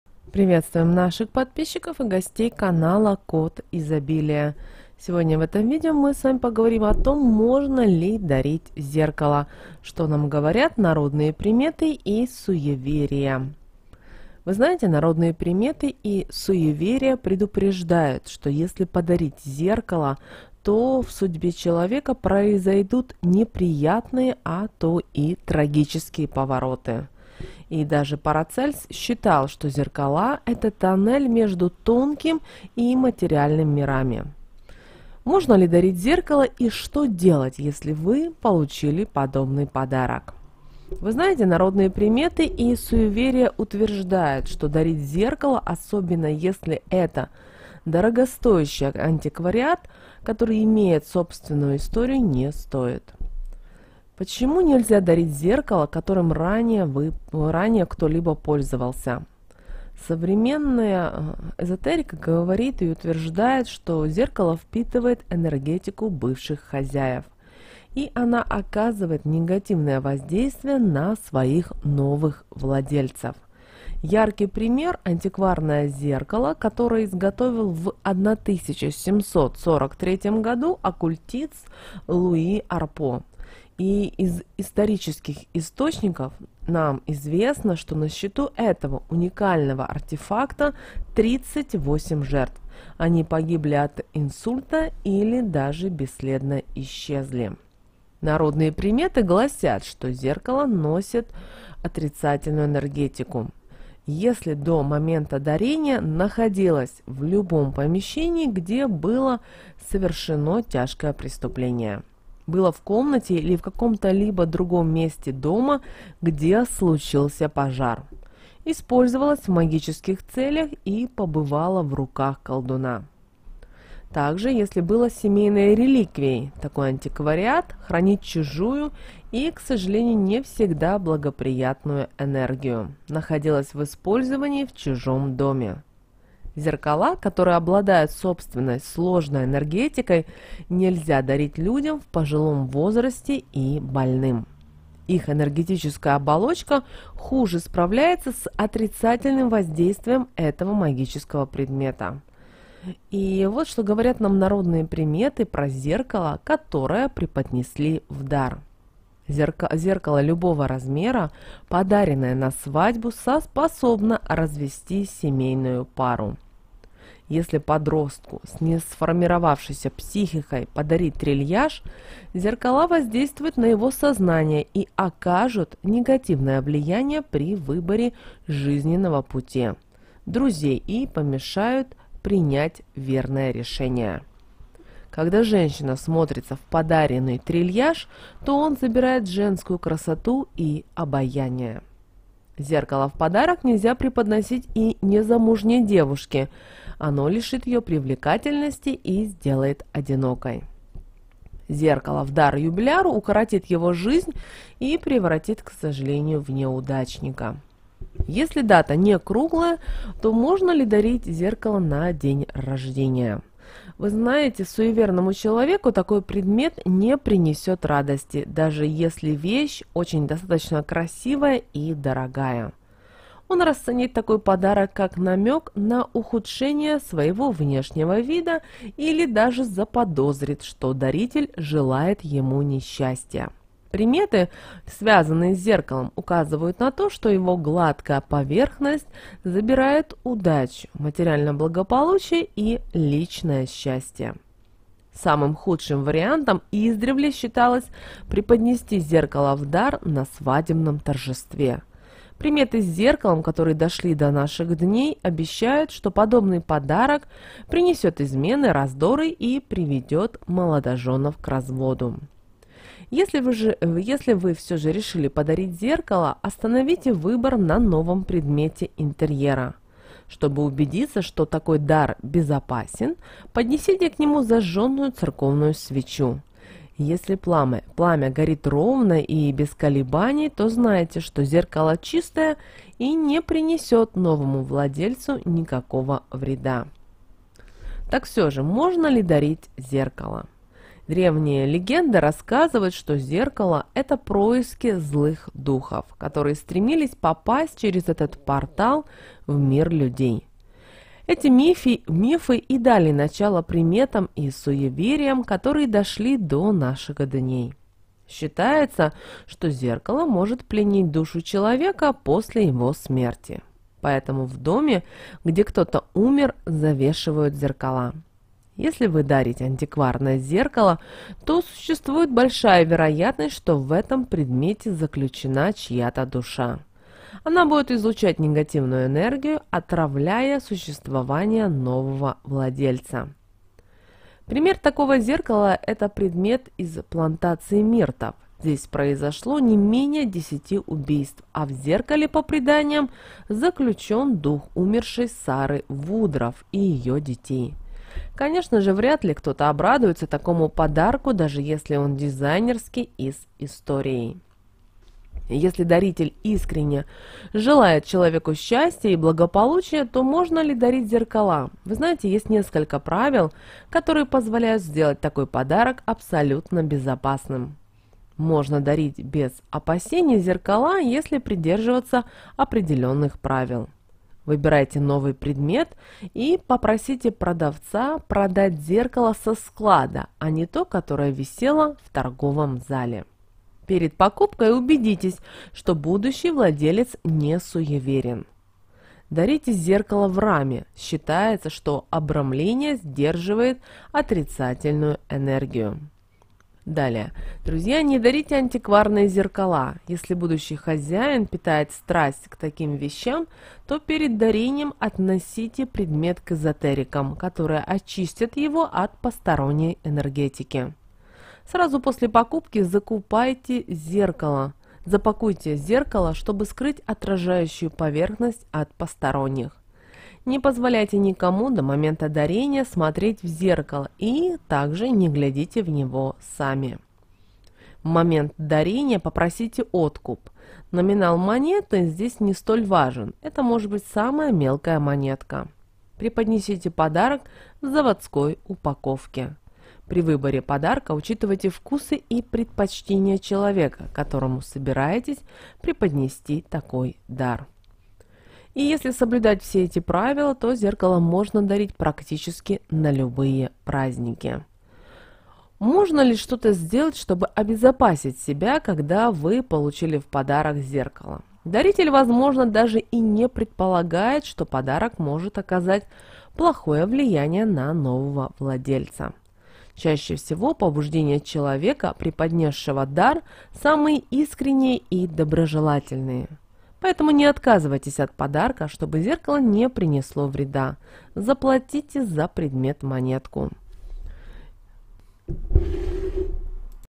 Если подарить зеркало,в судьбе человека произойдут неприятные,ато и трагические повороты.Так ли это?
Если подарить зеркало, в судьбе человека произойдут неприятные, а то и трагические повороты. Так ли это? Народные приметы про зеркало. Что нельзя делать с зеркалом

Канал "КОТ ИЗОБИЛИЯ" - эзотерический видео канал, каталог интересных и познавательных видео.